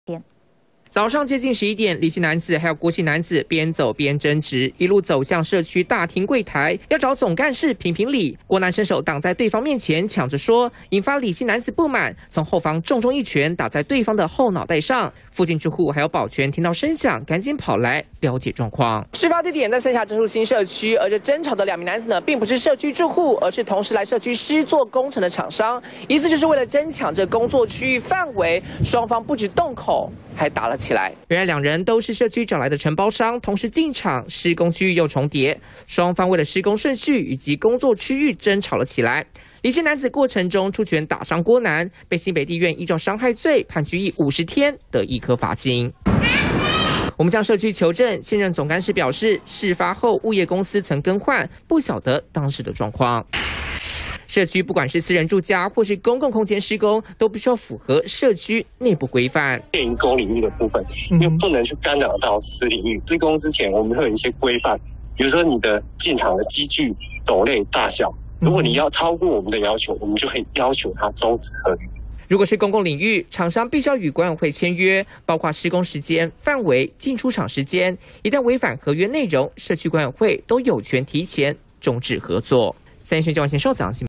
「為何你先做」搶進施工區 承包商大打出手│@politics_setn
兩家廠商進駐社區施工，怎麼最後演變成全武行!新北市三峽一處社區，日前有兩家承包商因為施工期還有地點重複，兩家廠商為了搶"作業區"吵了起來，原本要找社區總幹事協調，但過程中李姓男子不滿被阻擋，一拳打向對方的後腦，挨打的郭姓男子提告傷害，新北地院審理後，判打人的男子拘役50天得易科罰金。

友邦連結區
◎ 訂閱【三立新聞網】最新影片搶先看